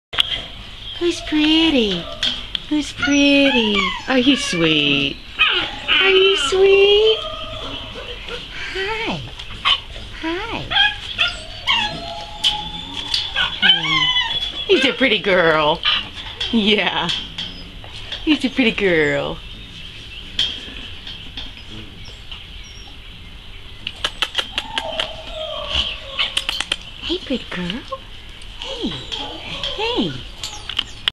Hound Mix Pup in Lancaster, SC
IN LANCASTER, SOUTH CAROLINA
Run # 8 - Female Hound Mix around 5 months old - ID # A19625209 - Hold released 04/17/2013